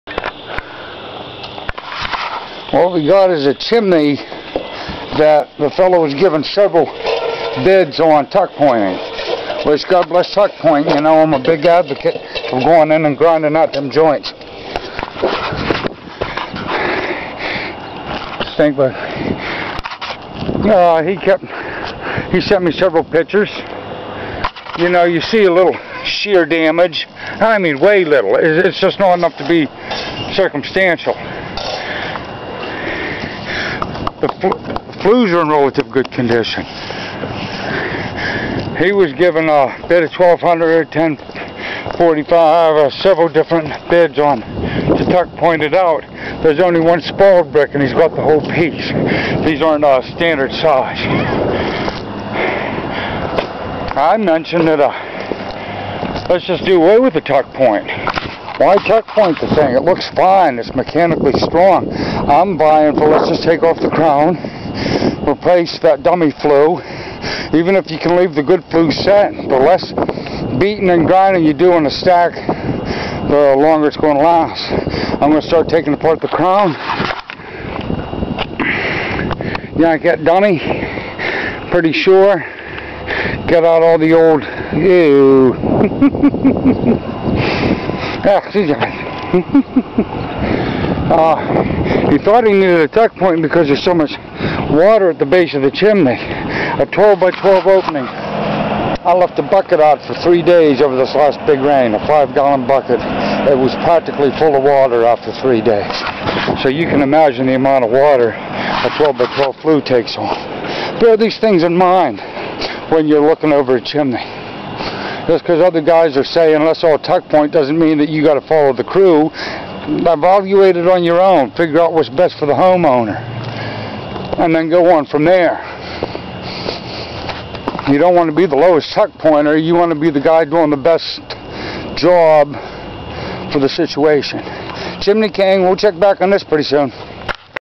chimneyking, The Beaver Job
Other contractors wanted to tuckpoint this stack.  I suggested a crown and caps to beat the water infiltration.